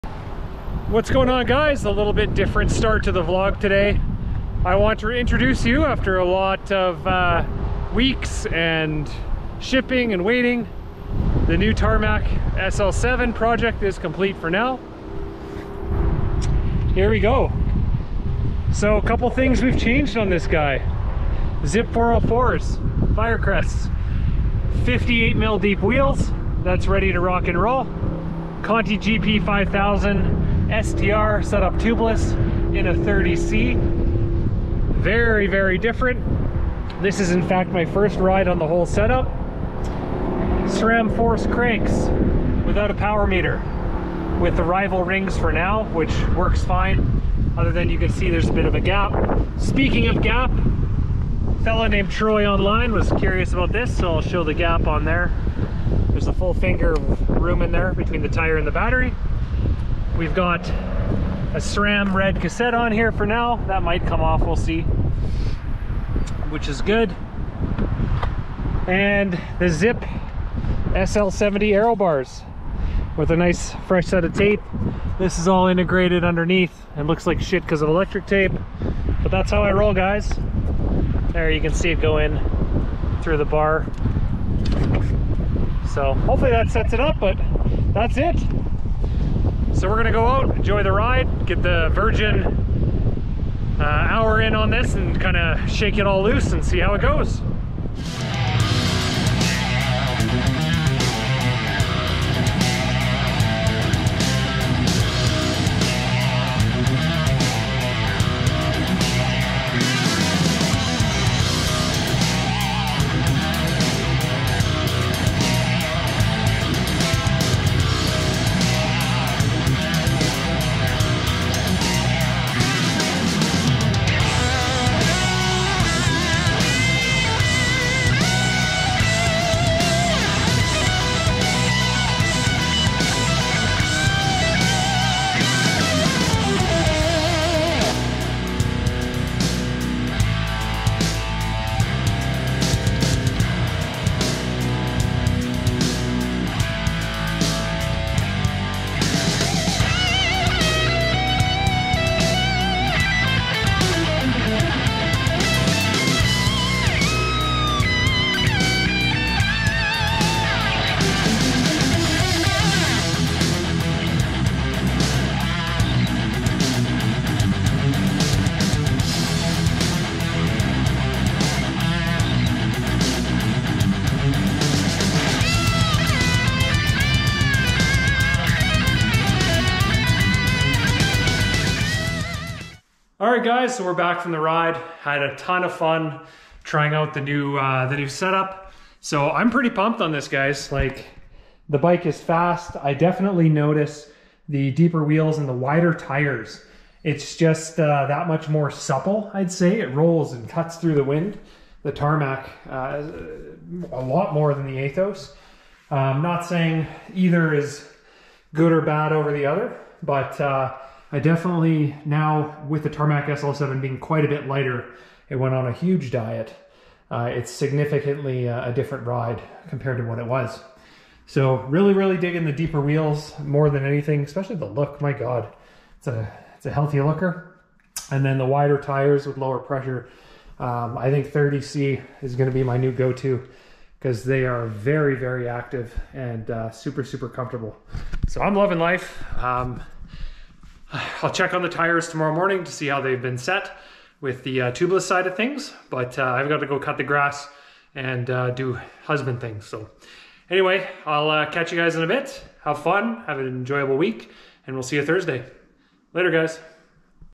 Specialized Tarmac SL7 - Project Complete
Showing you the completed Specialized Tarmac SL7 Project. Complete with Zipp 404 wheels, Zipp SL70 Aero Bars, Conti GP5000 S TR Tires, Sram AXS Drivetrain, and more.